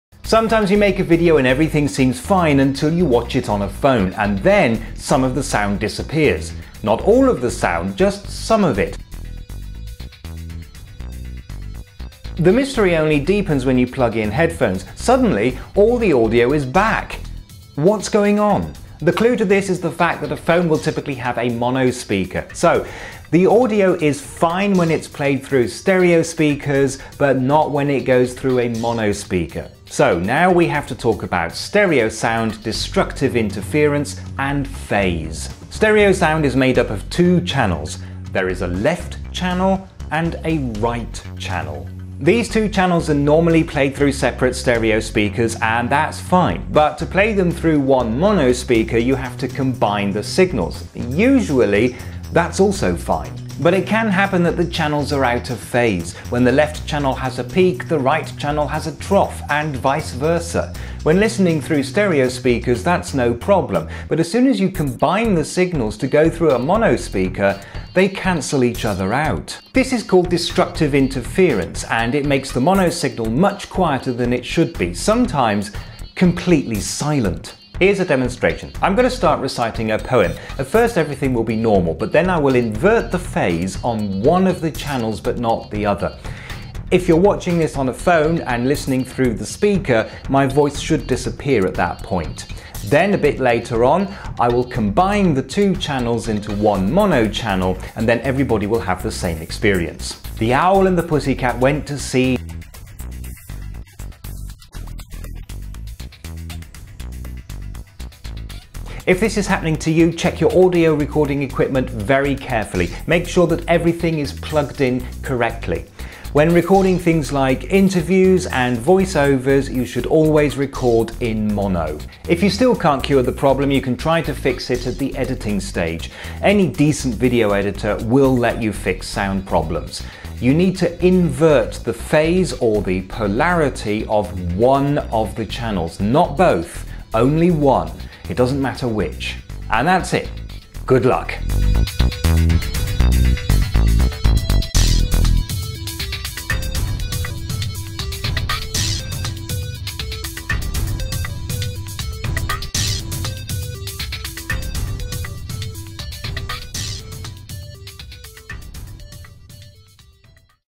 Inaudible audio: It's about phase
Sometimes parts of your audio are missing when you try to watch your videos on a phone. What's happening, and how can you fix it?

Follow the YT TC Blog for more of this, plus a heads-up on important bugs and new features: